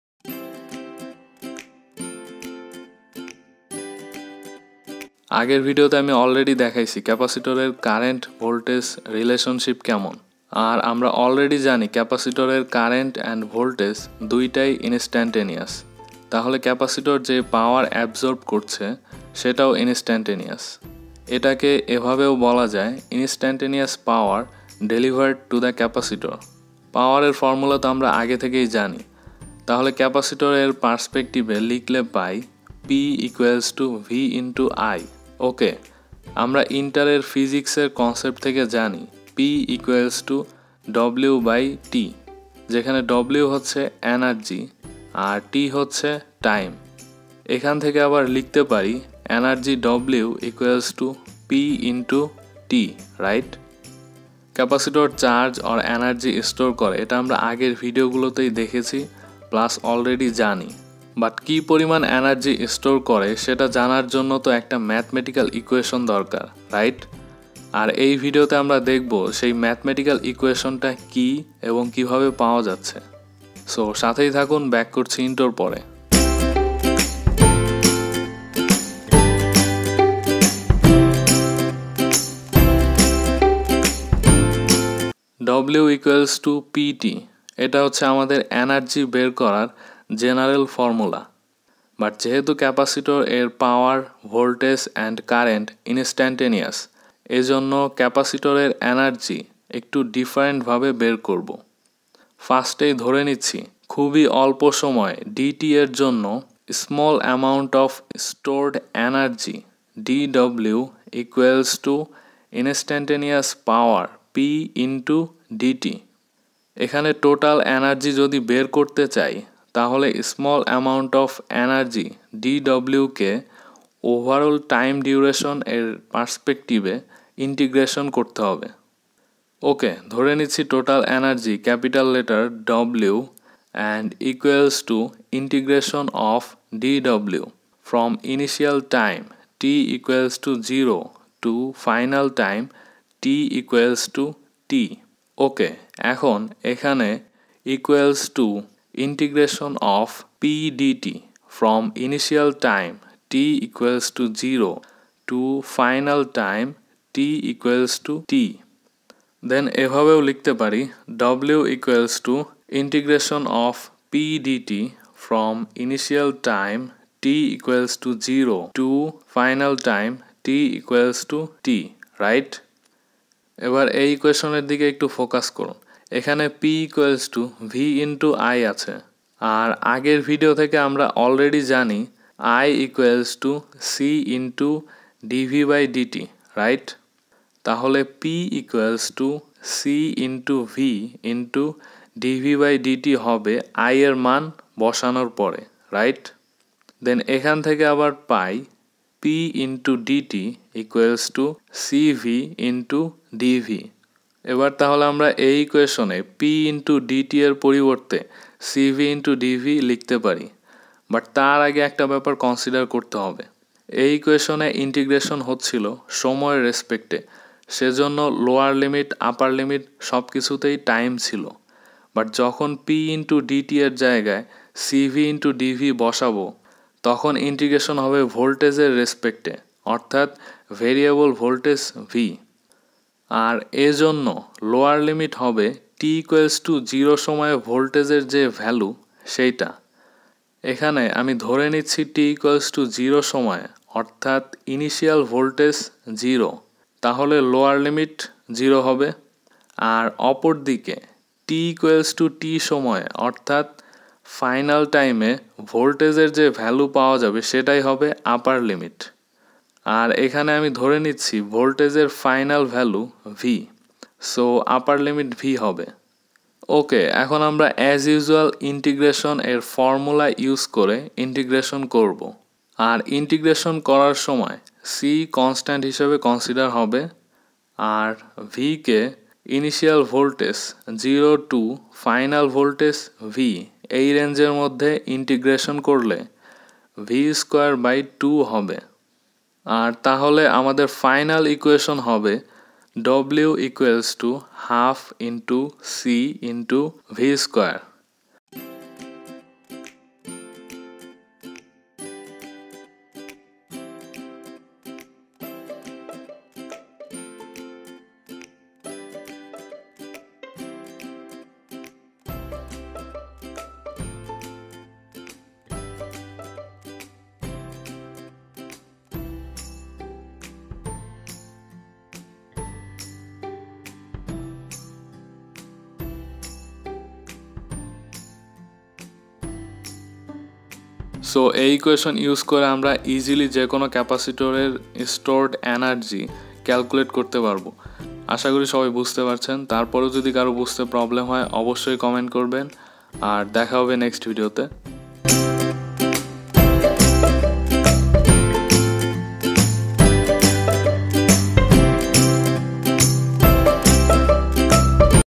Energy stored in Capacitor (bangla tutorial)
In this video we explain how to derive energy stored equation for capacitor in bangla. From this video you will learn how we get the equation for calculating stored energy in capacitor and how to calculate stored energy.
🚫Previous videos:
📎Why capacitor blocks dc and allows ac? (bangla tutorial) | Why capacitor is open circuit to dc?🔻